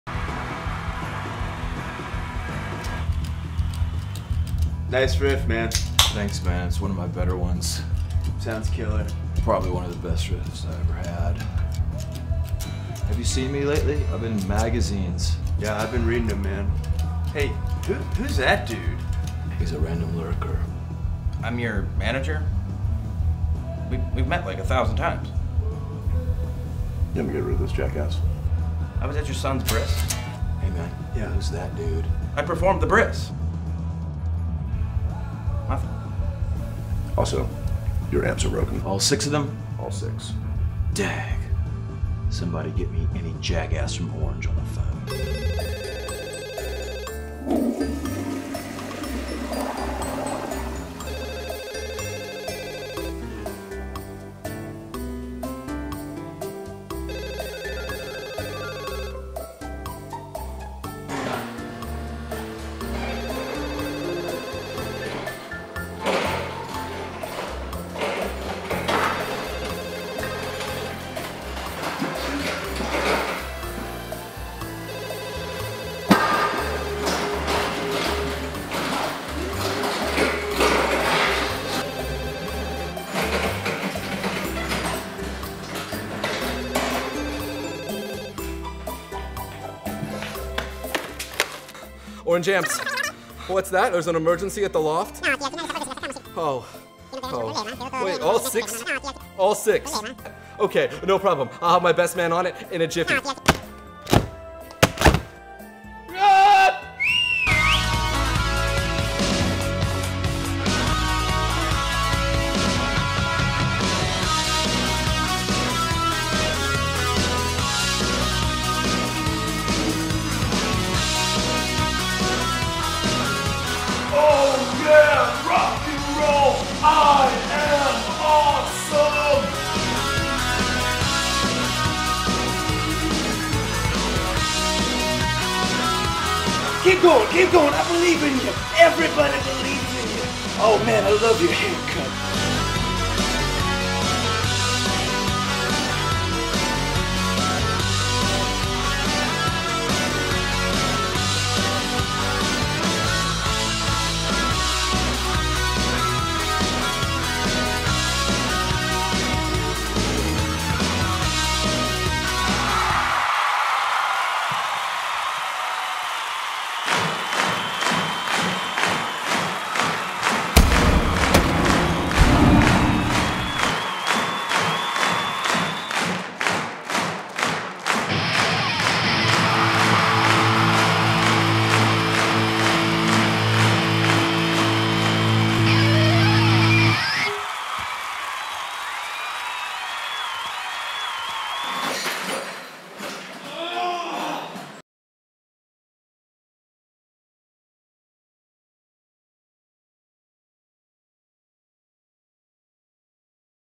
Orange Amps Saves The Day (Featuring Troy Sanders of Mastodon)
Troy Sanders of the explosive metal band Mastodon stars as a cocky rock star whose (non-Orange) amps have all broken just before a show. Orange sends its best "man" to save the day by delivering the Micro Terror to Troy just before he takes the stage!

The Micro Terror is Orange Amplifications smallest, most portable Terror amp yet, delivering a classic Orange sound way bigger than it looks and more than capable of giving enough stage volume to satisfy the mighty Mastodon.

To visit the Micro Terror page on the Orange Amps

A special thanks to Newmerica the creative video agency that collaborated with us to produce this video, Troy Sanders, Mastodon, Colin Miles, Darren Sanders, The Loft Atlanta, Criminal Records, and Bear the Dog. Music by The N.E.C. and Rump Posse